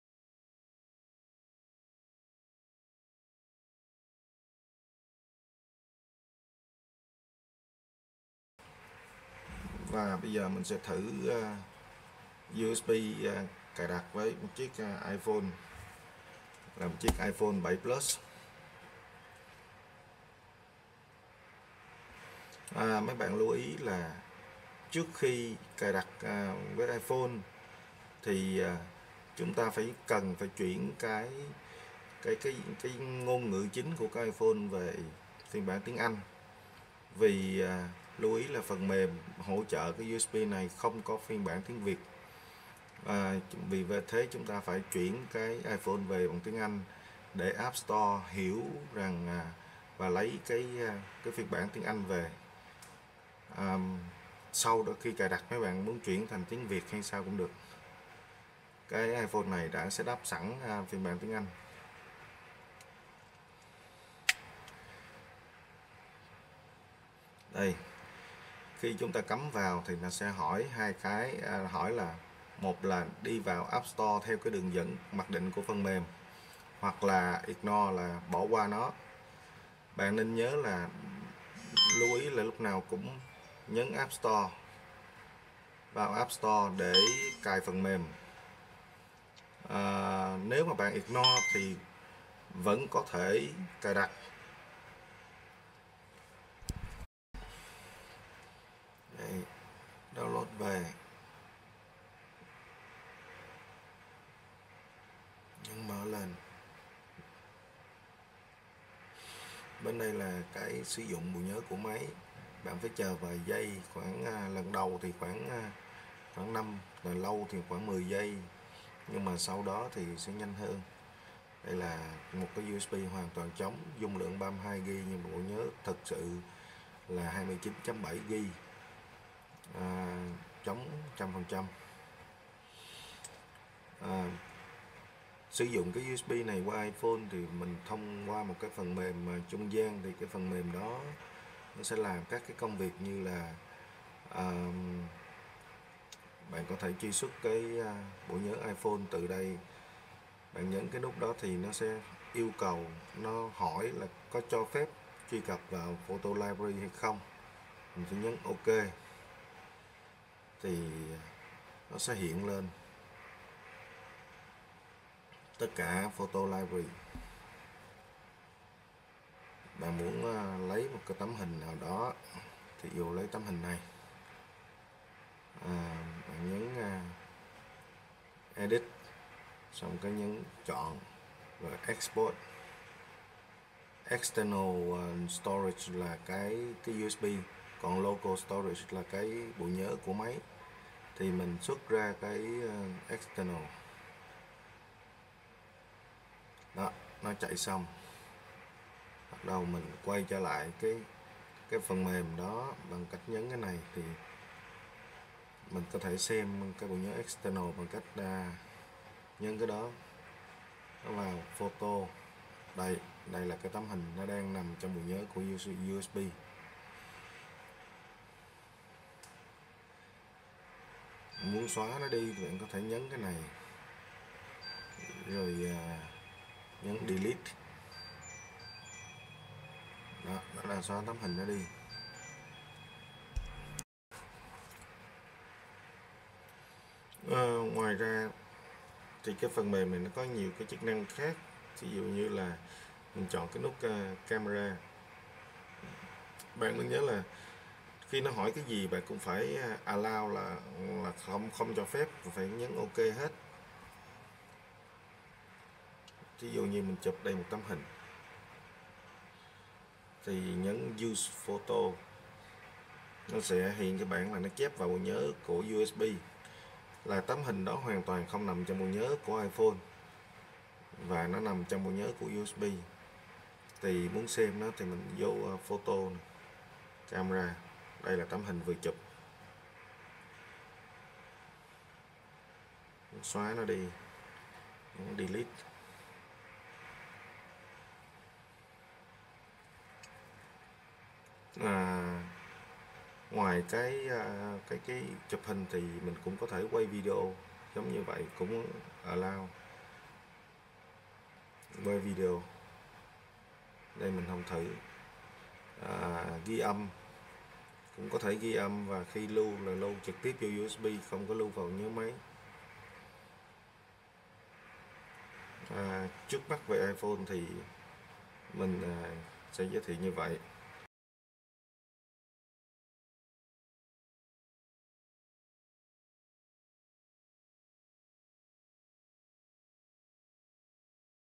Hướng dẫn cài đặt Smart USB đối với iPhone
Smart USB 3 trong 1 sử dụng cho iPhone - Android - Laptop. Duy chỉ đối với iPhone người dùng phải cài thêm 1 phần mềm trung gian, còn với Android chỉ cần cắm vào chọn mode OTG là nhận ra ngay.